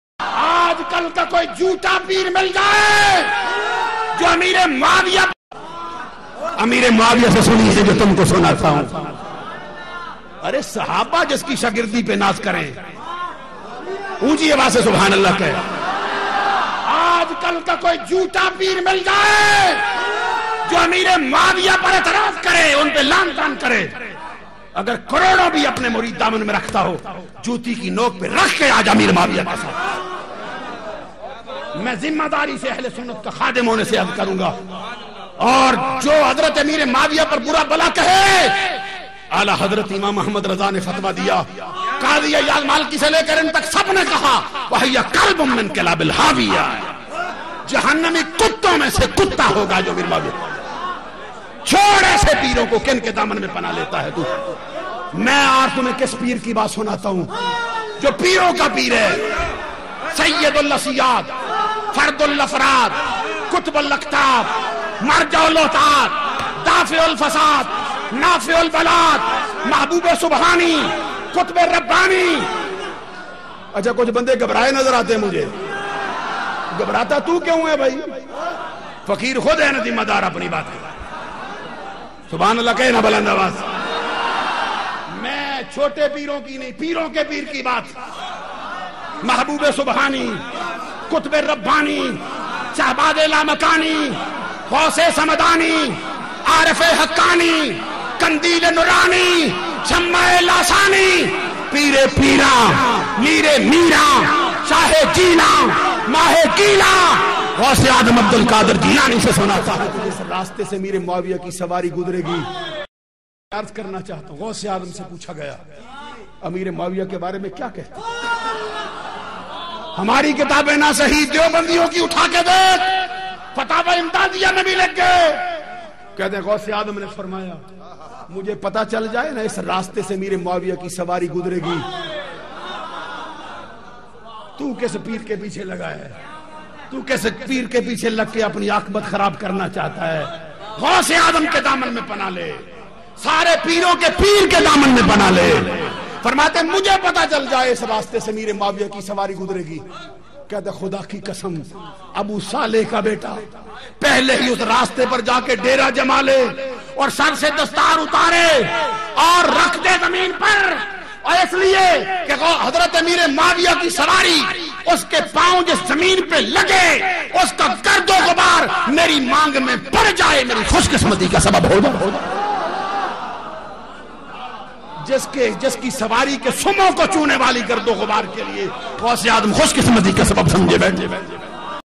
Hajrat amire ma'aviyah Ki shan me gustakhi karne wale Shia rafziyo suno Mufti Ansarul Qadri Sahab
Sahabi e Rasool Hajrat amire ma'aviyah Ki shan me gustakhi karne wale Shia rafziyo suno Mufti Ansarul Qadri Sahab (uk)